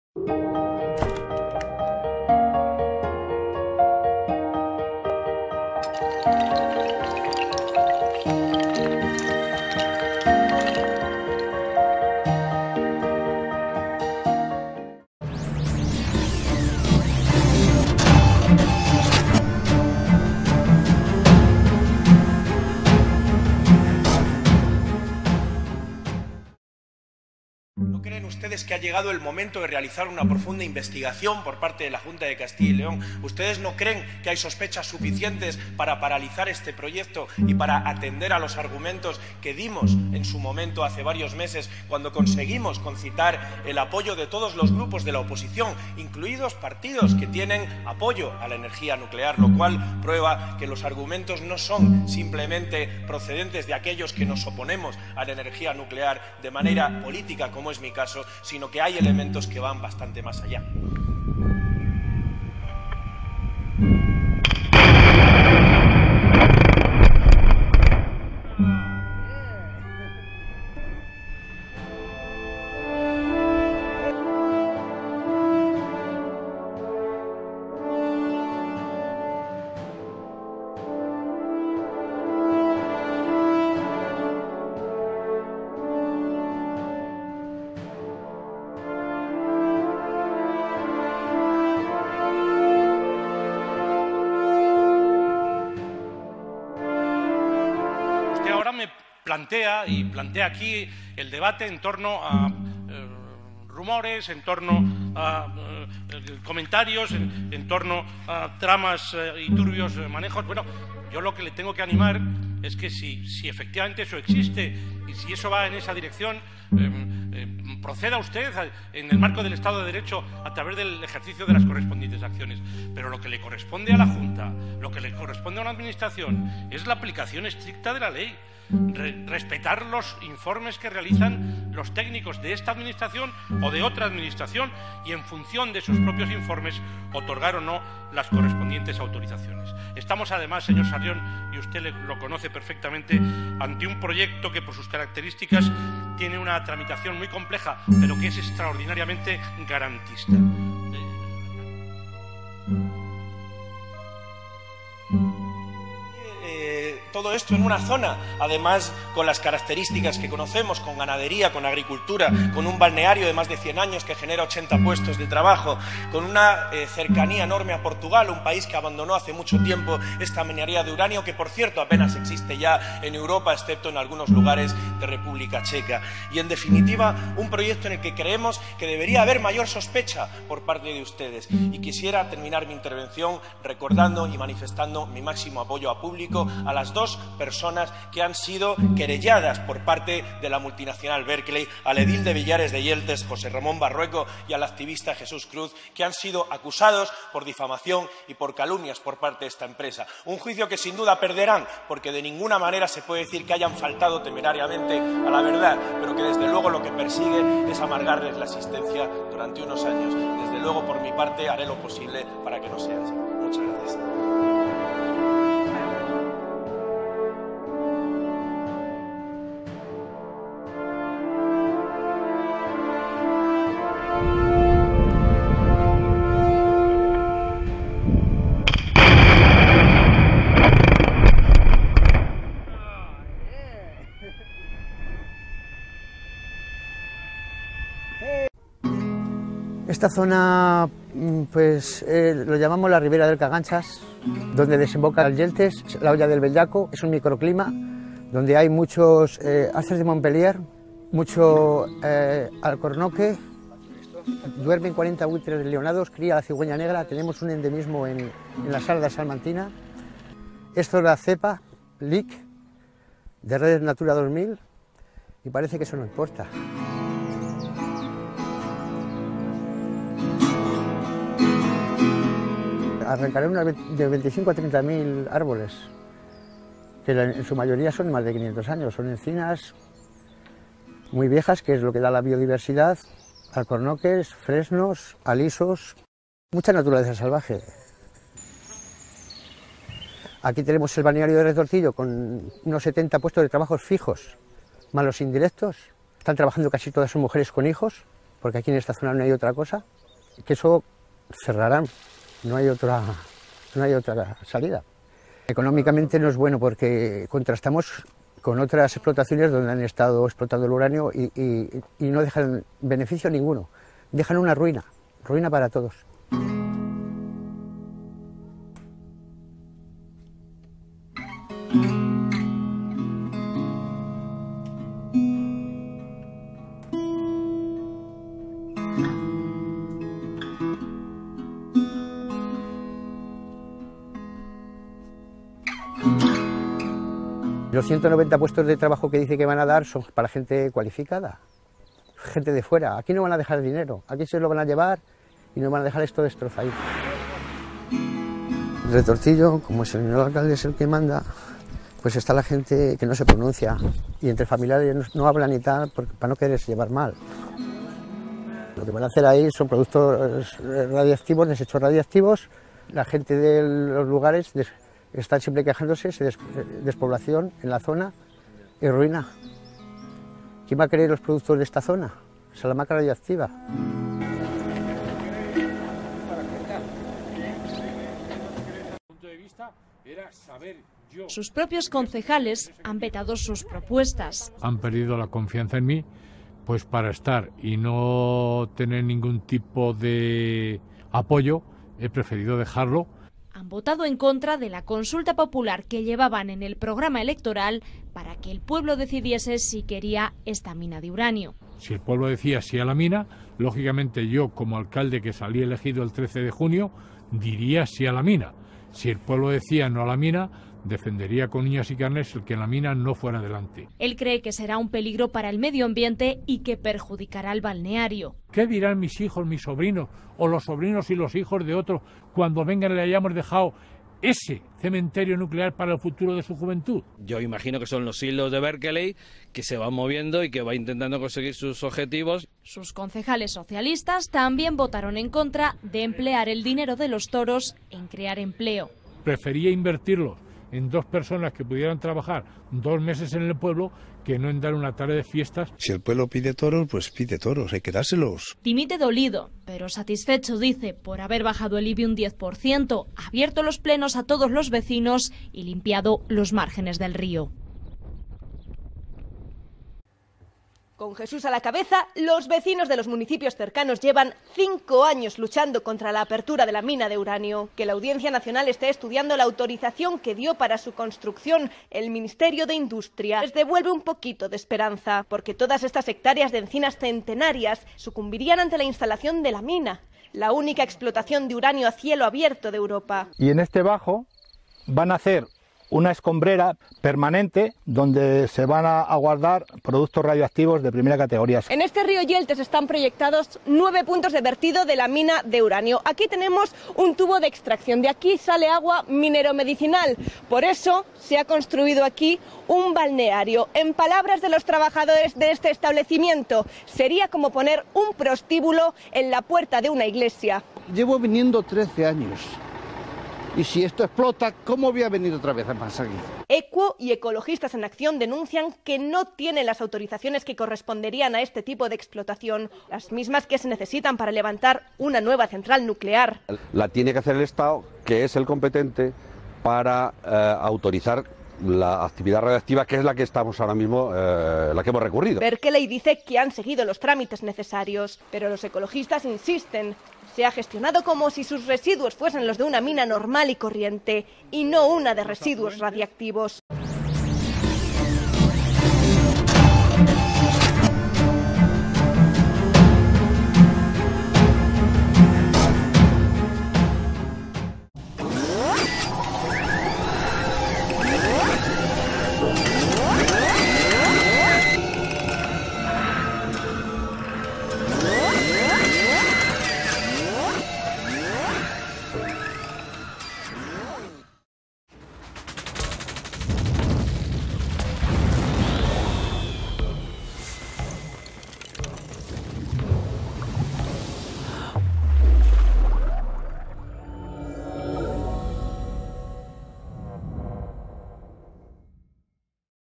LA MINA DE URANIO DE RETORTILLO (SALAMANCA-PAÍS LEONÉS)
COMPENDIO DE NOTICIAS DE MEDIOS DE COMUNICACIÓN ACERCA DEL PROYECTO A CAMPO ABIERTO DE UNA MINA DE URANIO, Y SUS NEFASTAS CONSECUENCIAS PARA EL PRESENTE Y FUTURO DE LAS COMARCAS CIRCUNDANTES AL ÁREA DE LA POBLACIÓN DE RETORTILLO (SALAMANCA-PAÍS LEONÉS).URANIO Y ENERGÍA NUCLEAR FUERA DE LAS TIERRAS LEONESAS.